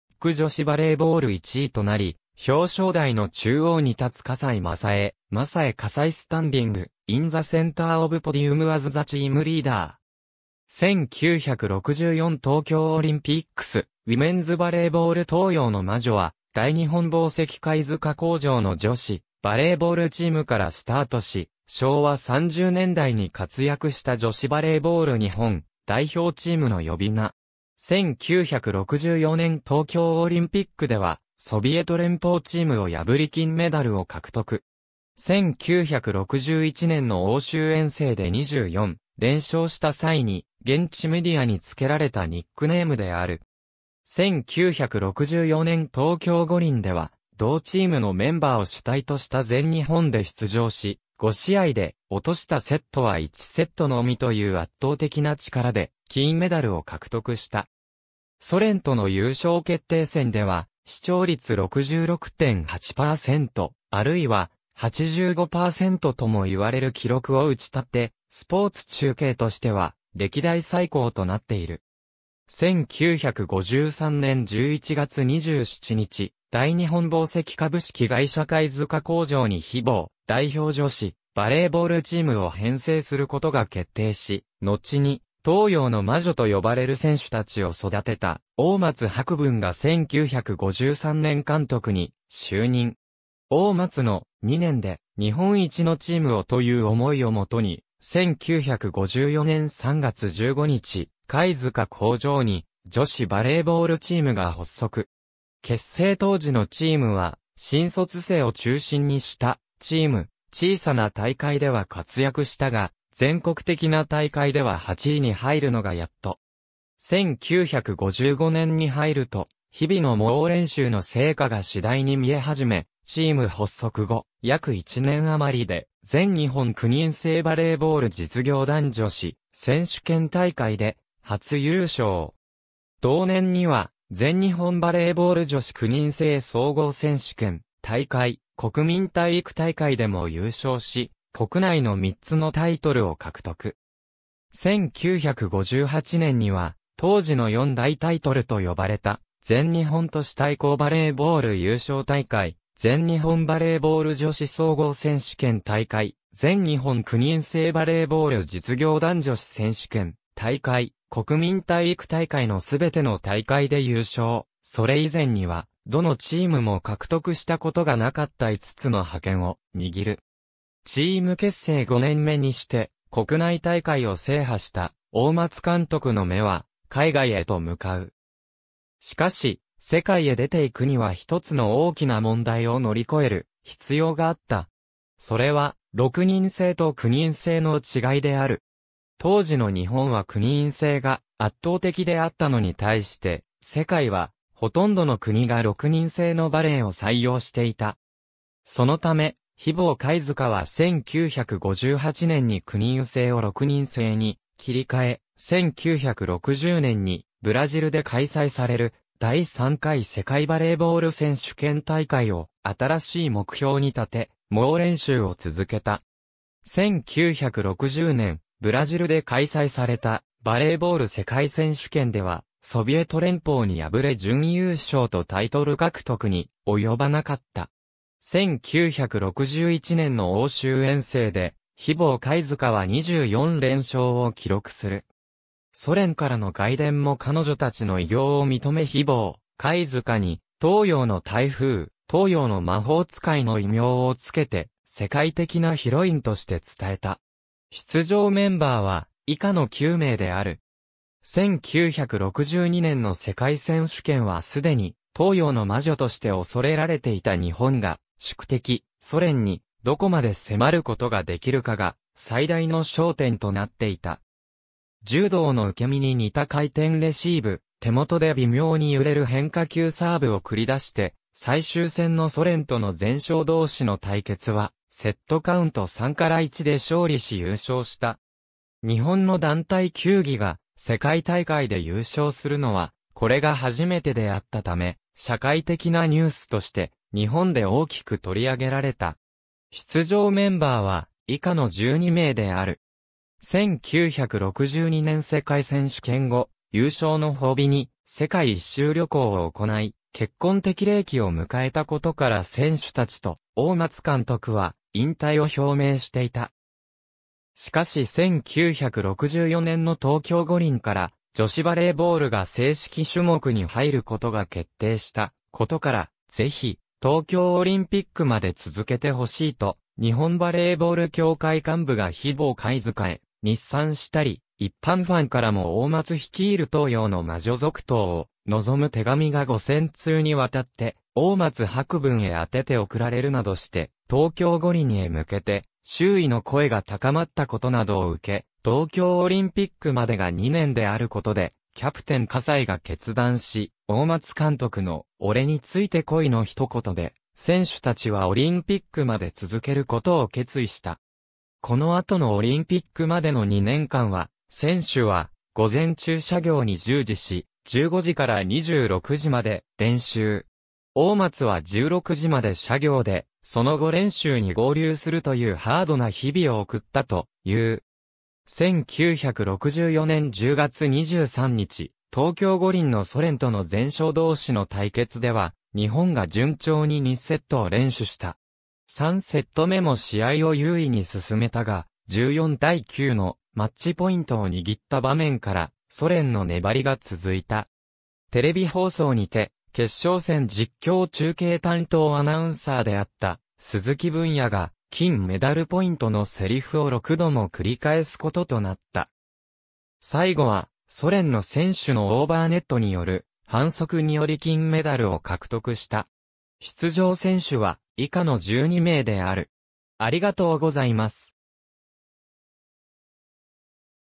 東洋の魔女
ユニチカ・フェニックス
ソビエトチームにスパイクを放つ磯辺サタ（Sata Isobe spiking ball against Soviet Union National Team, 1964 Tokyo Olympics Women's Volleyball） 東京オリンピック女子バレーボール１位となり、表彰台の中央に立つ河西昌枝（Masae Kasai standing in the center of podium as the team leader,
1964 Tokyo Olympics Women's Volleyball） 東洋の魔女（とうようのまじょ、英: Oriental Witches）は、大日本紡績（のちの、ユニチカ）貝塚工場の女子バレーボールチームからスタートし昭和30年代に活躍した女子バレーボール日本代表チームの呼び名。
1964年東京オリンピックでは、ソビエト連邦（現ロシア）チームを破り金メダルを獲得。
1961年の欧州遠征で24連勝した際に、現地メディアにつけられたニックネームである。
1964年東京五輪では同チームのメンバーを主体とした全日本で出場し、5試合で落としたセットは1セットのみという圧倒的な力で金メダルを獲得した。
ソ連との優勝決定戦では視聴率66.8%（ビデオリサーチ調べ、関東地区）、あるいは、85%ともいわれる記録を打ち立て、スポーツ中継としては歴代最高となっている。
1953年11月27日、大日本紡績株式会社貝塚工場に日紡代表女子バレーボールチームを編成することが決定し、のちに「東洋の魔女」とよばれる選手たちを育てた大松博文が1953年監督に就任。
大松の「2年で日本一のチームを」という思いをもとに、1954年3月15日、貝塚工場に女子バレーボールチーム（通称「日紡貝塚」）が発足。
結成当時のチームは新卒生を中心にしたチーム、小さな大会では活躍したが、全国的な大会では8位に入るのがやっと。
1955年に入ると、日々の猛練習の成果がしだいに見えはじめ、チーム発足後約1年余りで、全日本9人制バレーボール実業団女子選手権大会で初優勝。
同年には、全日本バレーボール女子9人制総合選手権大会、国民体育大会でも優勝し、国内の3つのタイトルを獲得。
1958年には、当時の4大タイトルと呼ばれた全日本都市対抗バレーボール優勝大会、全日本バレーボール女子総合選手権大会、全日本9人制バレーボール実業団女子選手権大会、国民体育大会の全ての大会で優勝、それ以前にはどのチームも獲得したことがなかった5つの覇権（女子総合選手権大会は6人制、9人制の両方で優勝したため）をにぎる。
チーム結成5年目にして国内大会を制覇した大松監督の目は、海外へとむかう。
しかし、世界へ出ていくにはひとつの大きな問題を乗り越える必要があった。
それは、6人制と9人制の違いである。
当時の日本は9人制が圧倒的であったのに対して、世界はほとんどの国が6人制のバレーを採用していた。
そのため、日紡貝塚は1958年に9人制を6人制に切りかえ、1960年にブラジルで開催される第3回世界バレーボール選手権大会を新しい目標にたて、猛練習を続けた。
1960年、ブラジルで開催されたバレーボール世界選手権（英語版：「1960 FIVB Volleyball Women's World Championship」）では、ソビエト連邦に敗れ準優勝とタイトル獲得に及ばなかった。
1961年の欧州遠征で日紡貝塚は24連勝を記録する。
ソ連からの外電も彼女たちの偉業を認め日紡貝塚に「東洋の台風」、「東洋のまほうつかい」の異名をつけて世界的なヒロインとして伝えた。
出場メンバーは以下の9名である。
1962年の世界選手権はすでに東洋の魔女として恐れられていた日本が宿敵・ソ連にどこまで迫ることができるかが最大の焦点となっていた。
柔道の受け身に似た回転レシーブ、手元で微妙に揺れる変化球サーブを繰り出して最終戦のソ連との全勝同士の対決はセットカウント3-1で勝利し優勝した。
日本の団体球技が世界大会で優勝するのはこれが初めてであったため、社会的なニュースとして日本で大きく取り上げられた。
出場メンバーは以下の12名である。
1962年世界選手権後、優勝の褒美に世界一周旅行を行い、結婚適齢期を迎えたことから選手達と大...